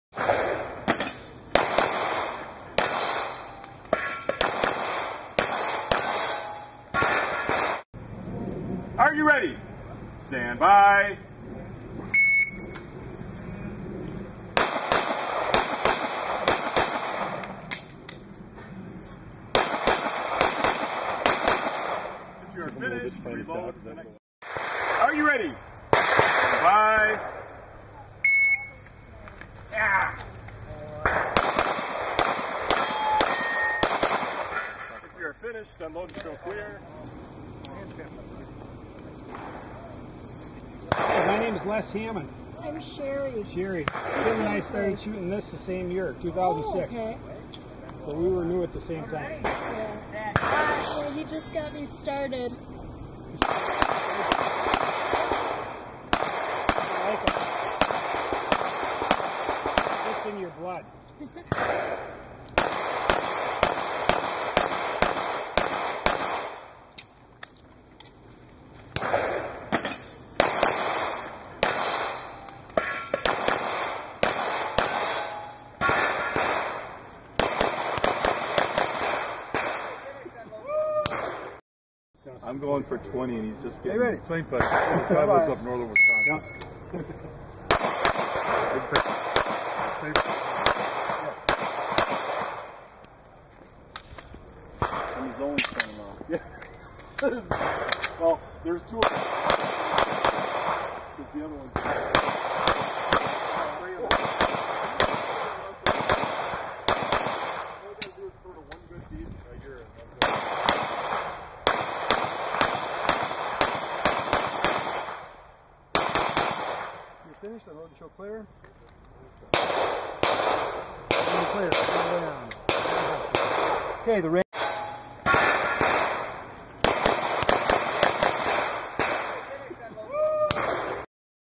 Peter Nielsen OGC 7/21/11
I am shooting at the Thursday night OGC USPSA league match 7/21/11/